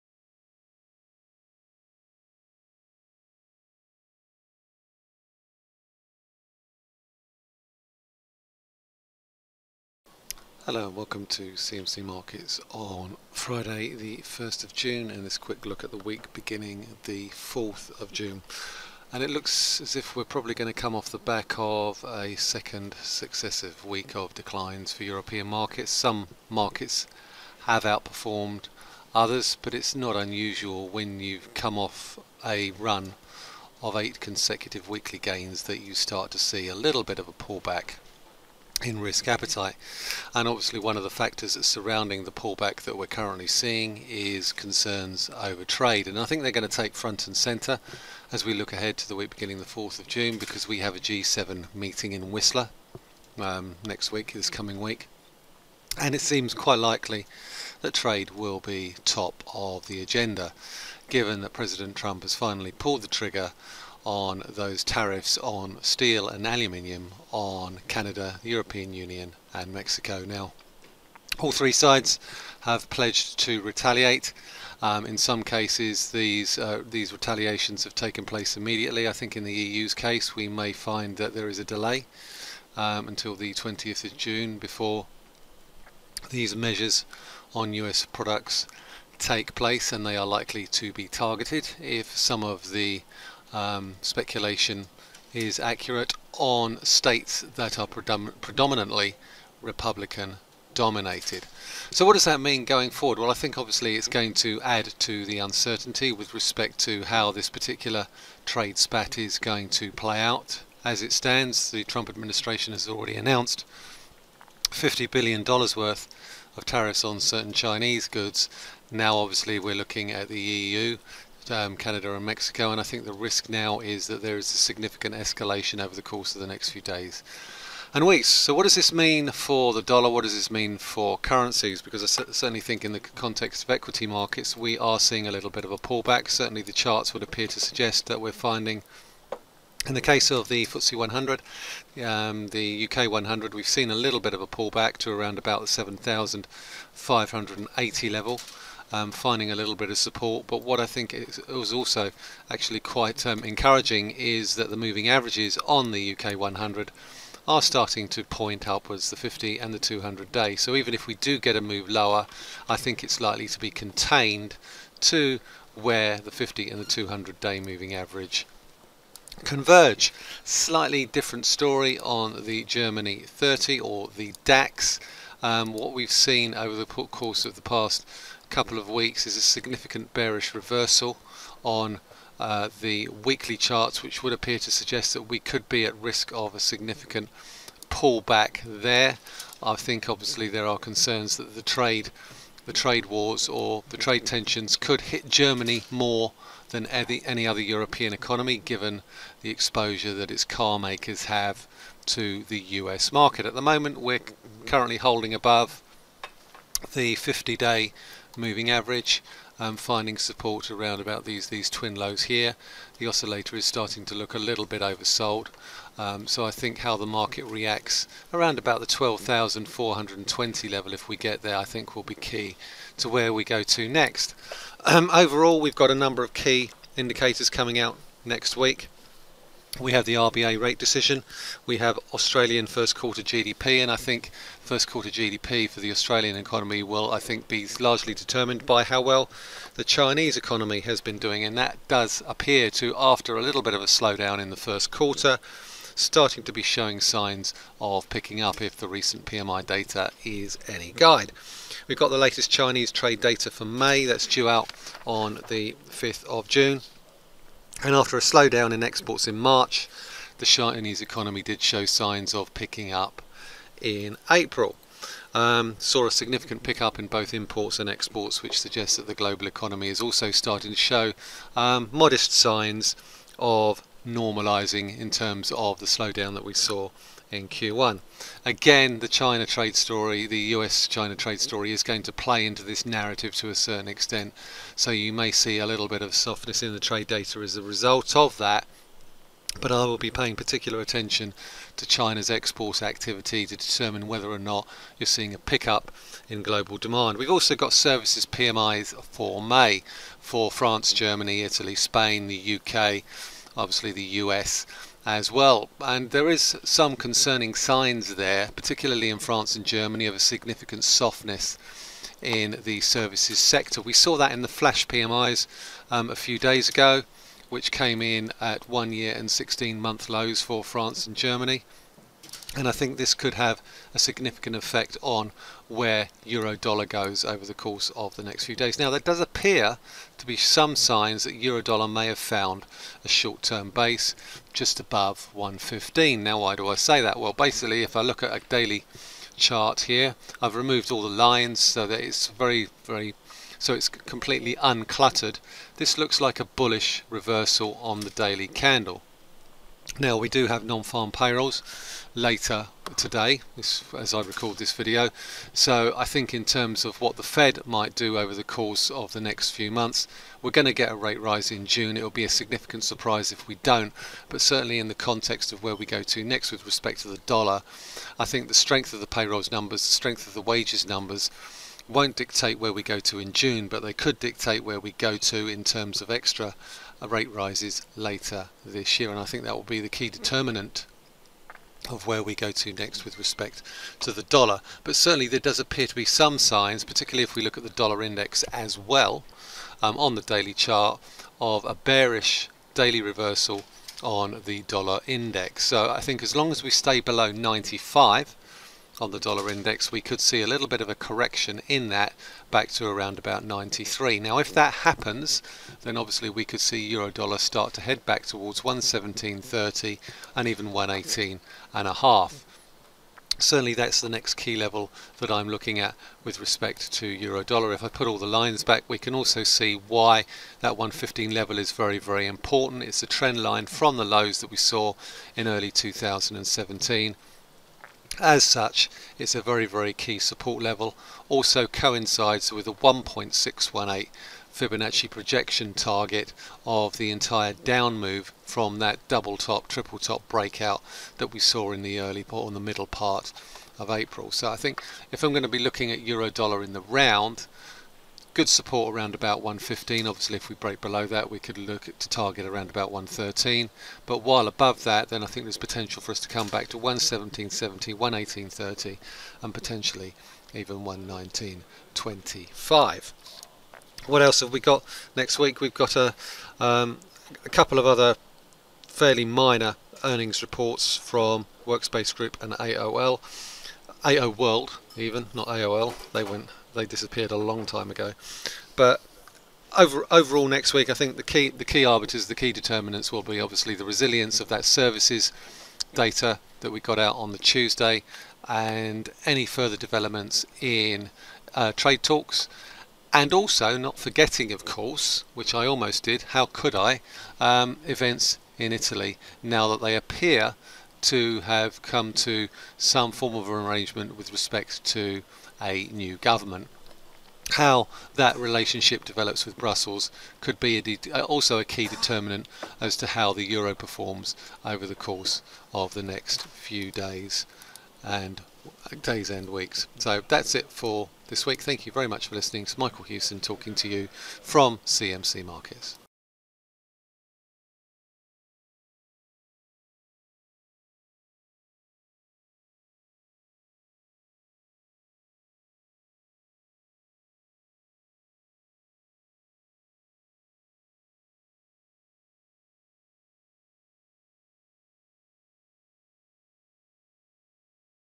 Trade, services and G7 - 2 June 2018
Chief Market Analyst, Michael Hewson, looks back at this week's price action and ahead to the next steps on trade, tariffs, G7, Italy and services PMIs.

Trade the financial markets, including forex, indices, commodities, shares and treasuries.